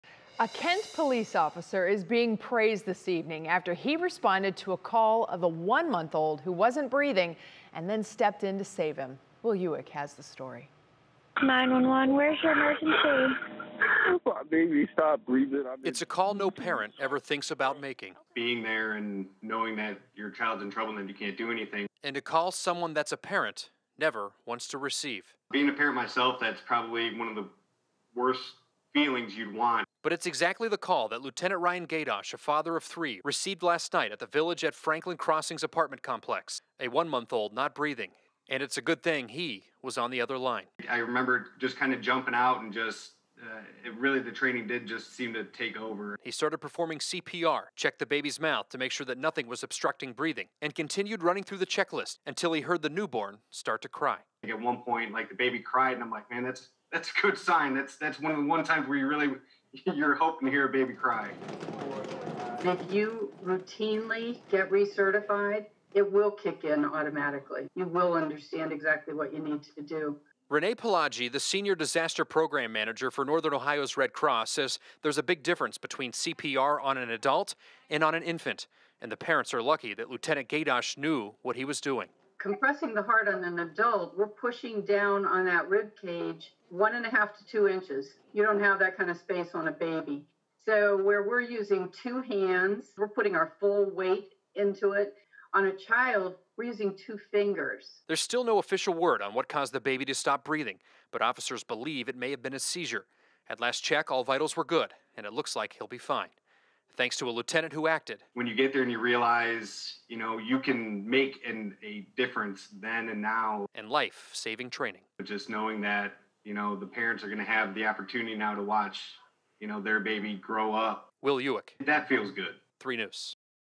Kent police officer praised after saving 1-month-old boy using CPR
Authorities say the incident occurred just before 1 a.m. The boy appears to be on track for a full recovery.